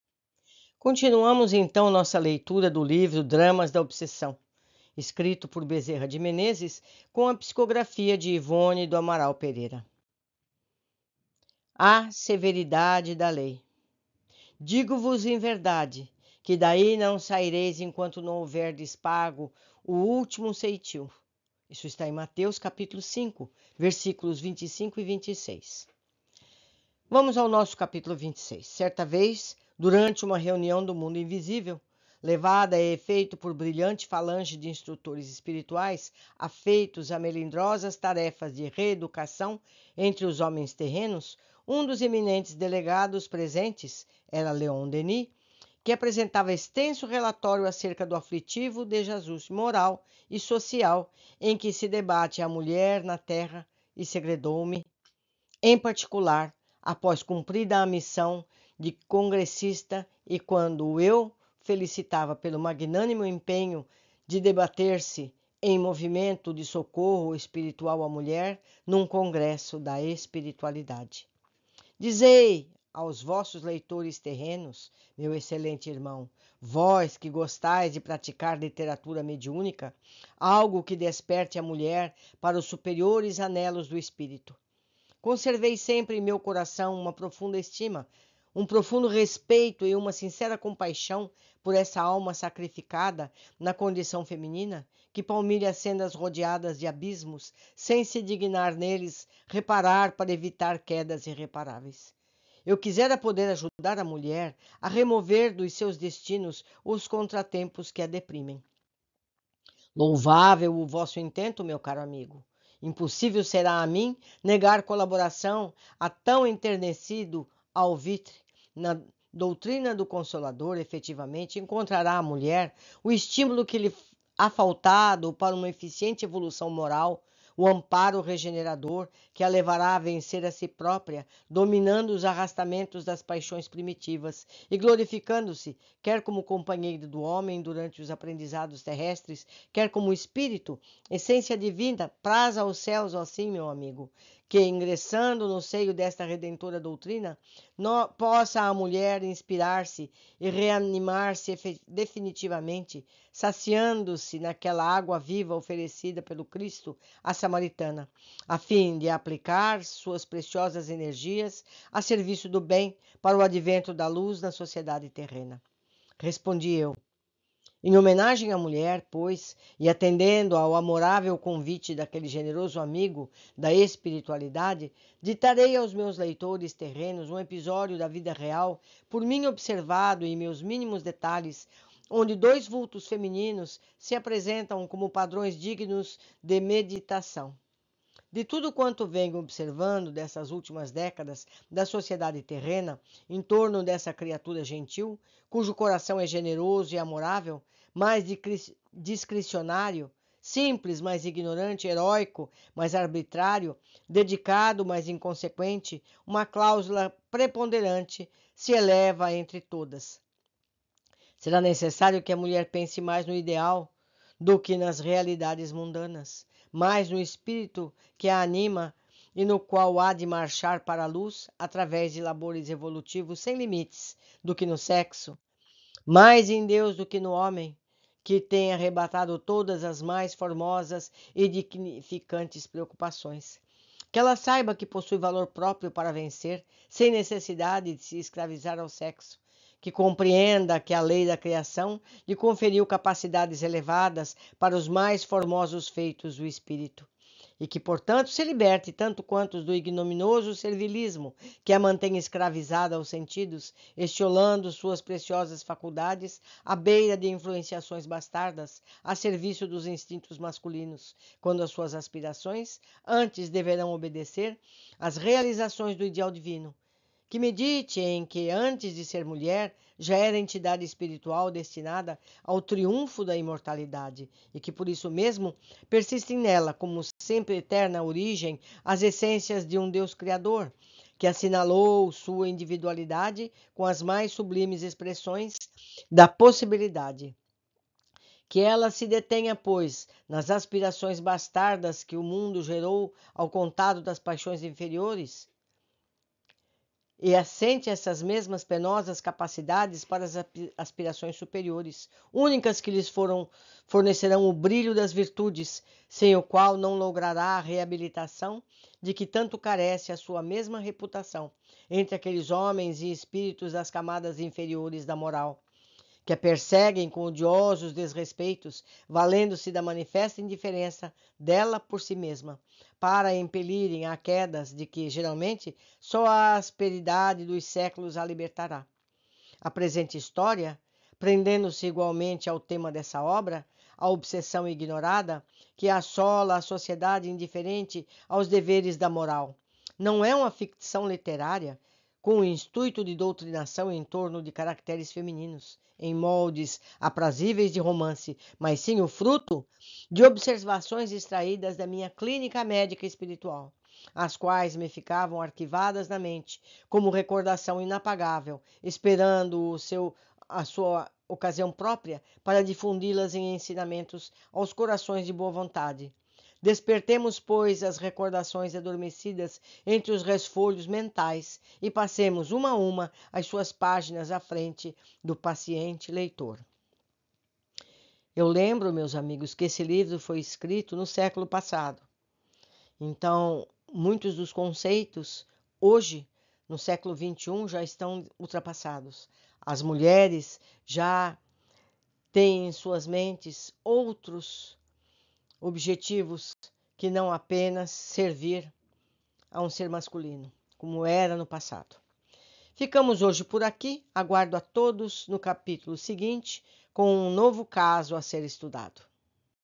26 -DRAMAS DA OBSESSÃO- SEVERIDADE DA LEI - CAP 26 -AUDIOLIVRO COMENTADO
Dramas da obsessão, o Espírito Bezerra de Menezes apresenta duas histórias baseadas em casos reais com preciosos ensinamentos sobre a lei de causa e efeito (ou de ação e reação), que aperfeiçoam nossas almas em razão do amor. Com psicografia da médium Yvonne A. Pereira, as cativantes narrativas nos ajudam a compreender o fenômeno da obsessão e as ligações profundas que podem ser estabelecidas entre as criaturas por meio do ódio e da vingança, além dos possíveis séculos de sofrimento até que se dissolvam as desavenças ao toque suave do perdão.